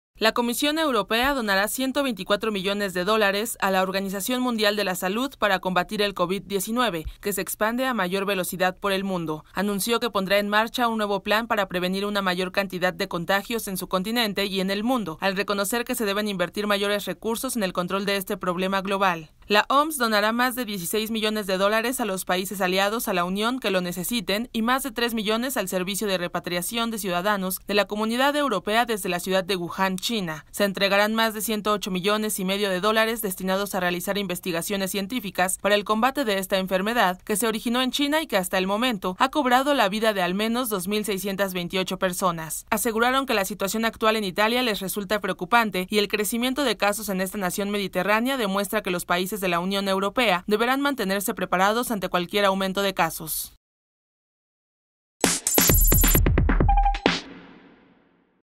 Comisión Europea donará 124 mdd a la OMS para combatir Covid 19
La Comisión Europea donará 124 millones de dólares a la Organización Mundial de la Salud para combatir el Covid-19.